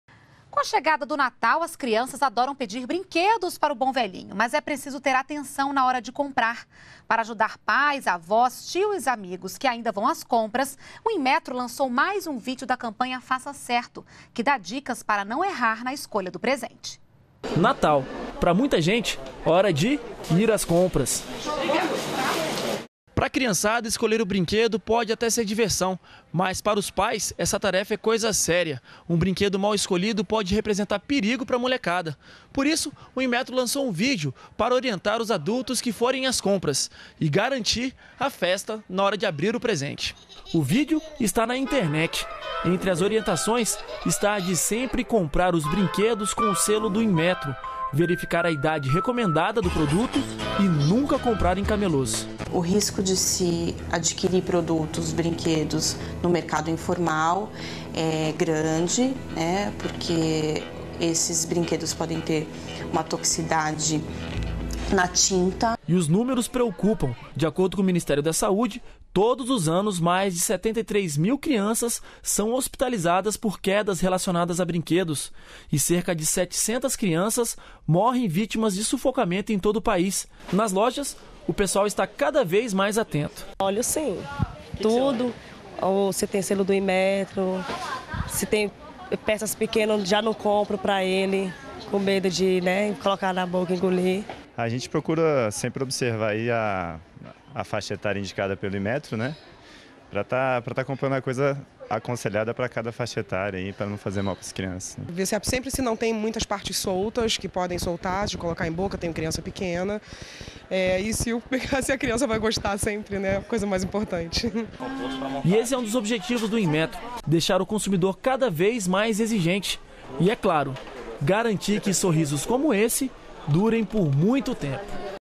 Inmetro lança vídeo para ajudar a comprar presentes para crianças
NBR NOTÍCIAS - 21.12.12: Com a chegada do Natal, as crianças adoram pedir brinquedos
para o bom velhinho. Mas é preciso ter atenção na hora de comprar os presentes. Para ajudar pais, avós, tios e amigos que ainda vão às compras o Inmetro lançou mais um vídeo
da campanha "Faça Certo". Publicação dá dicas para não errar na escolha do presente.